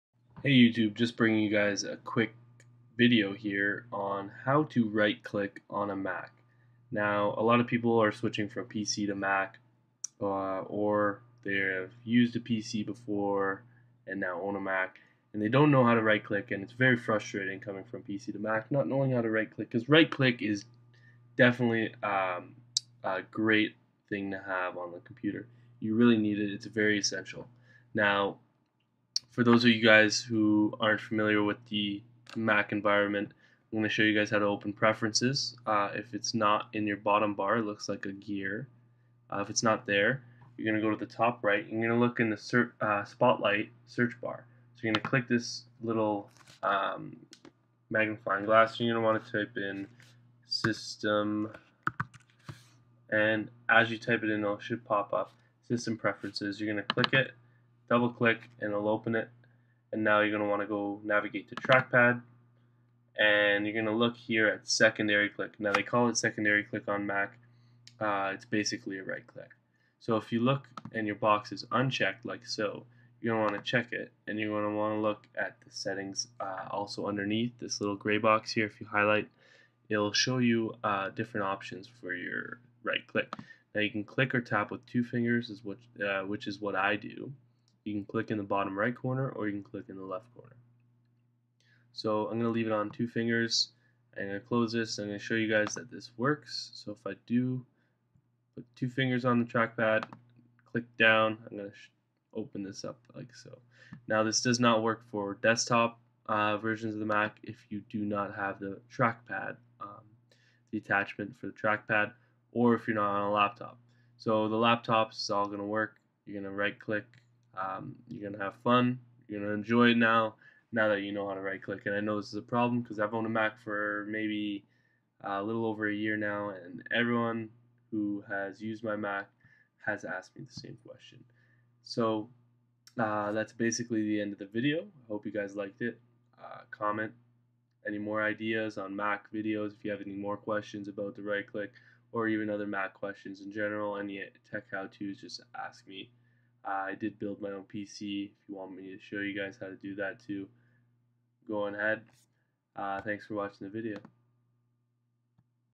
How to right click on mac
Quick little tutorial for the new mac users who are frustrated and cant figure out the right click option on mac.